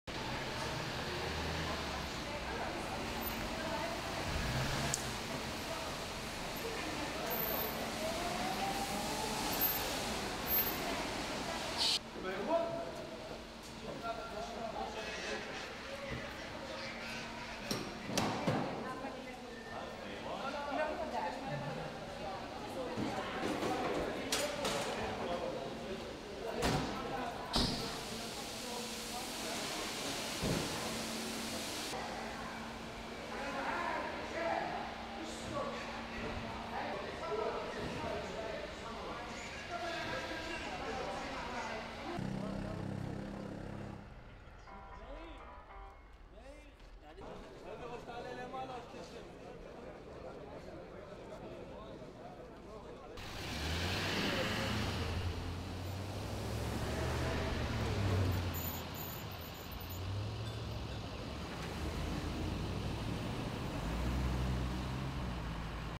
Aftermath of attack on synagogue, ambulances take wounded to hospital
(18 Nov 2014) Two Palestinians stormed a Jerusalem synagogue on Tuesday, attacking worshippers praying inside with knives, axes and guns, and killing four people before they were killed in a shootout with police, officials said.
Several of those injured in the attack were rushed to a hospital in ambulances.
Police said six people were wounded in the attack, including two police officers.
The attack, in Jerusalem's ultra-Orthodox Har Nof neighbourhood, was the deadliest in Jerusalem in years and is bound to increase fears of sustained violence in the city, already on edge amid soaring tensions over a contested holy site.
Israeli police described the assault as a "terrorist attack" and said the two assailants were apparently Palestinians from east Jerusalem.